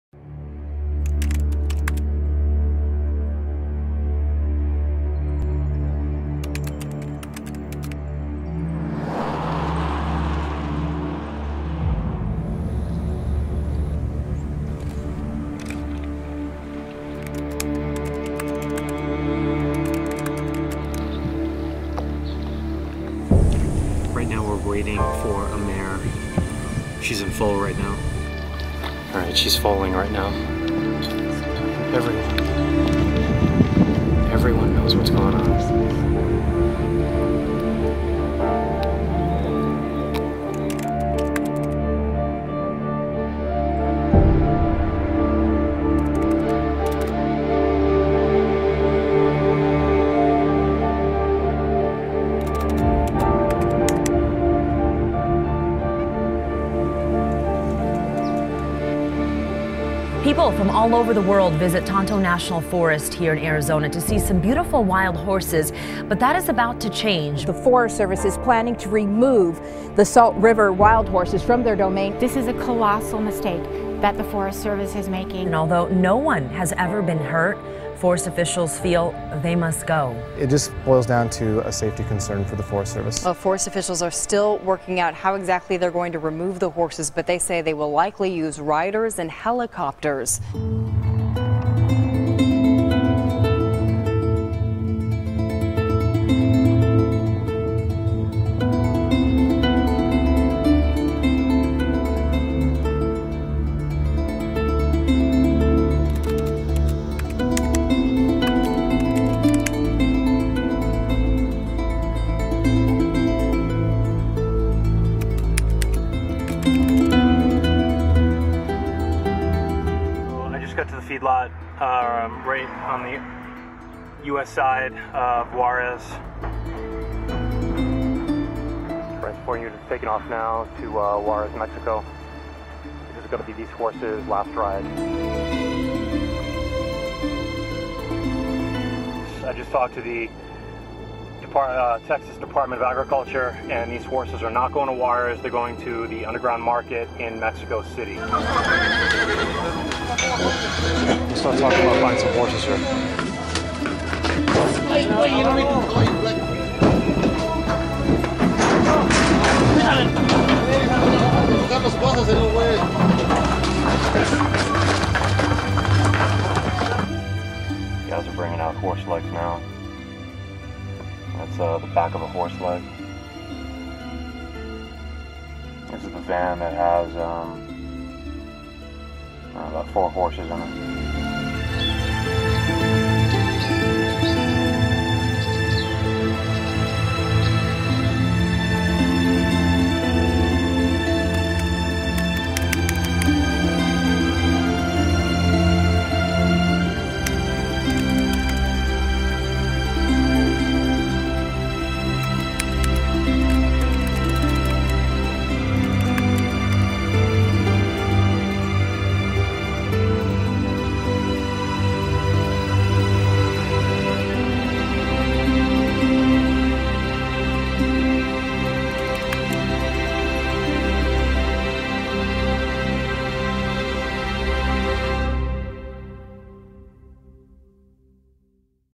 ARM Salt River Wild Horses
A short video on the beauty of the Salt River Wild Horses that grace the banks of the Salt River in Mesa, Arizona.

On Monday, 3rd August 2015, the Tonto National Park Forest Services  placed a small announcement in the local newspaper regarding a last minute roundup. The Salt River Wild Horse Management Group             (Which Animal Recovery Mission/ ARM is encompassed in) has been working around the clock with petitions, politicians, celebrity and advocacy support to ensure these majestic horses are not rounded up .
This video is an overview of ARM's work in the field with the Salt River Horses and our work in Mexico where many wild horses end up. Educational and spreading awareness, we encourage every viewer to support by signing petitions, calling the congressmen and senators and of course, sharing this around the world. This is what will save the wild horses !